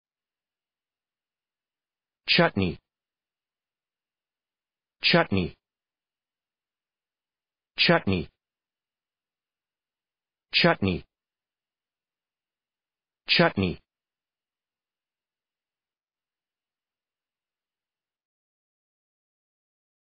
How to pronounce chutney
chutney American English pronunciation. How to pronounce chutney correctly. How to say chutney in proper American English.